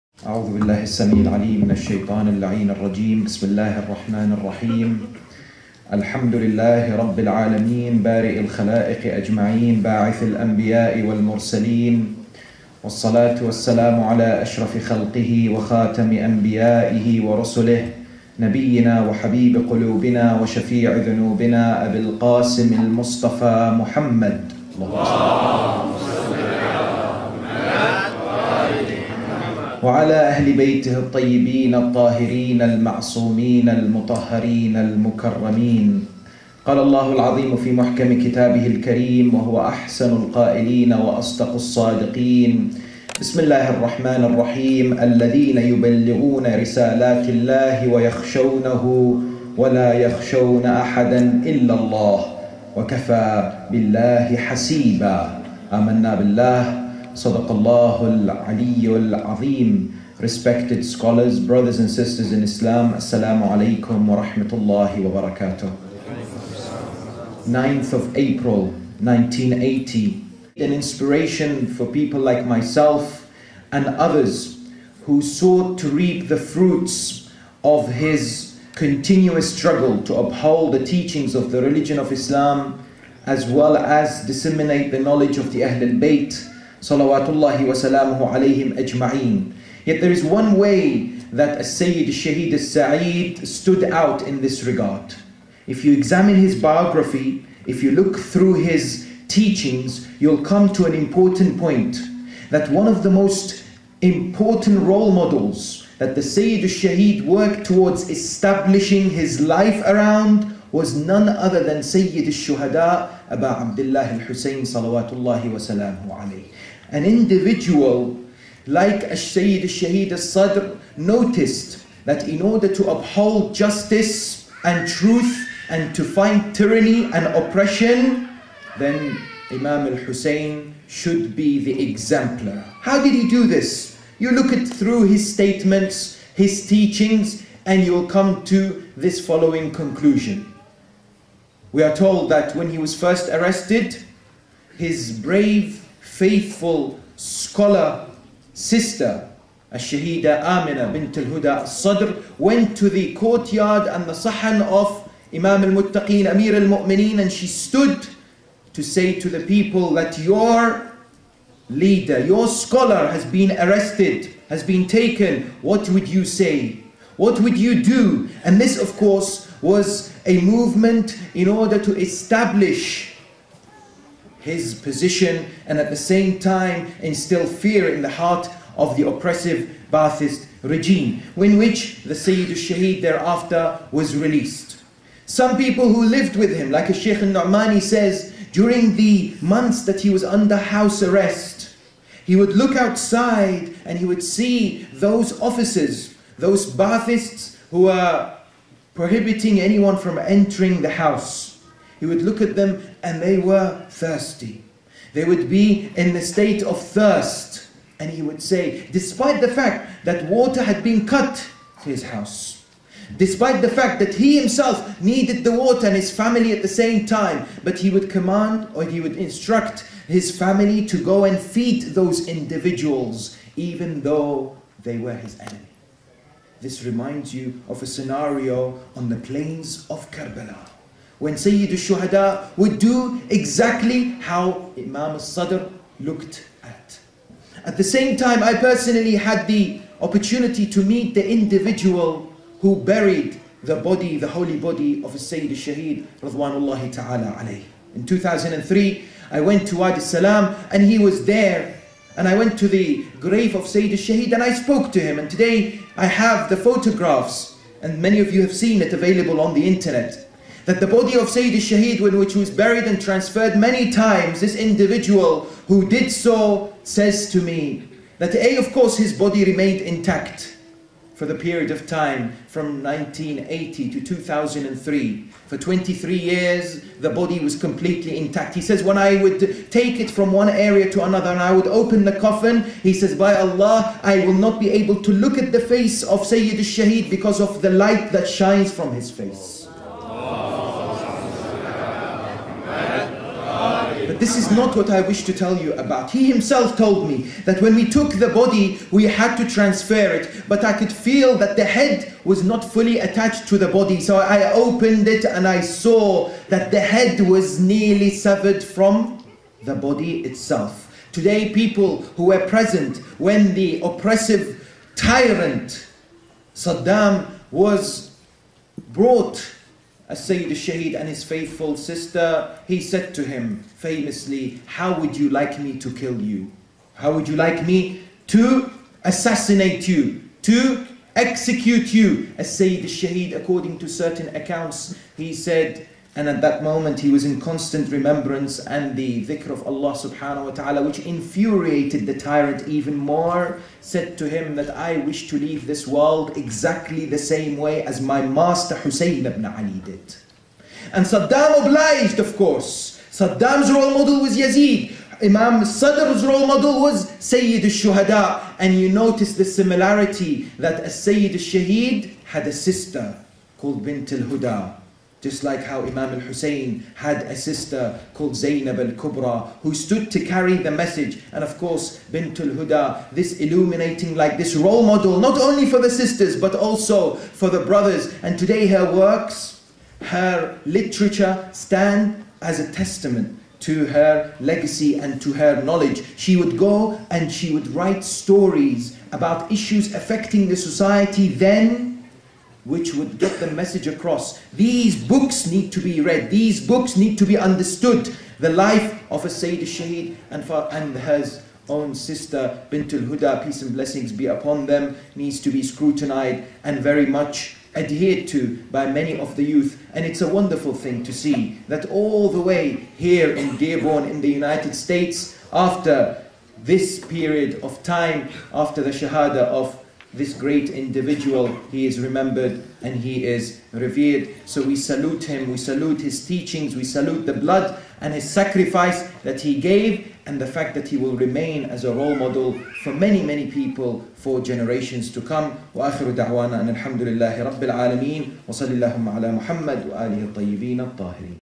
Sadr Program: Sheikh Mohammed Al-Hilli Lecture
Sadr Program @ Islamic House of Wisdom, Dearborn, Michigan
07 April, 2012
Commemorating the death of Shaheed Mohammad Baqir Al-Sadr